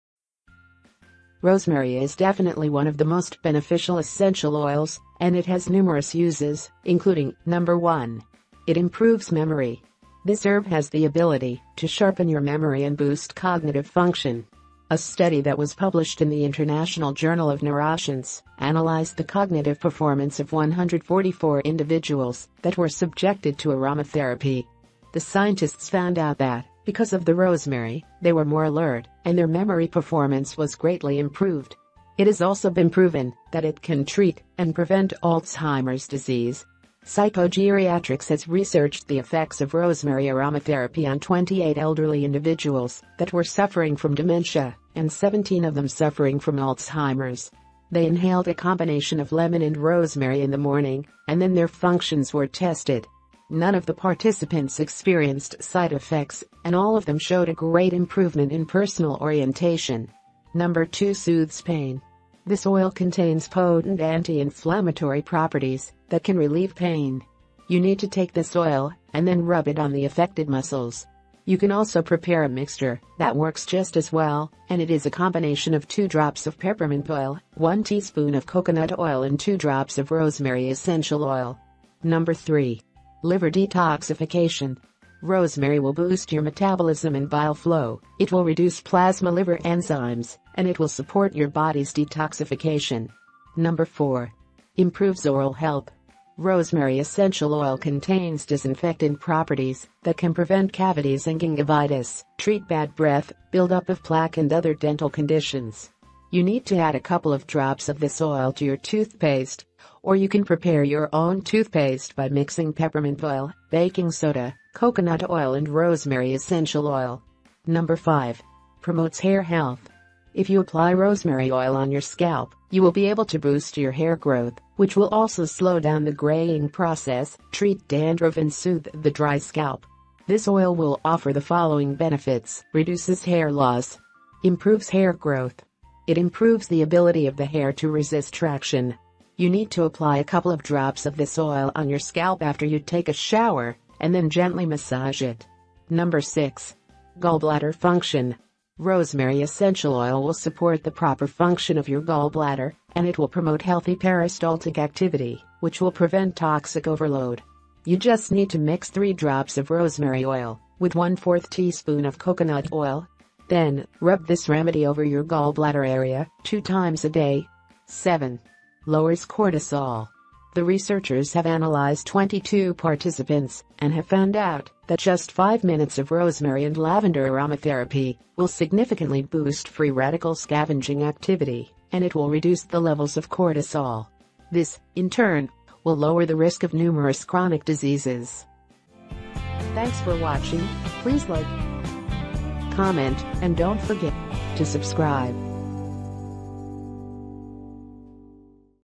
The amazing essential oil, that detoxifies your liver and boosts your memory
Rosemary is definitely one of the most beneficial essential oils, and it has numerous uses, including:.
Here this essential oil, that detoxifies your liver and boosts your memory

essential oil diffuser how to use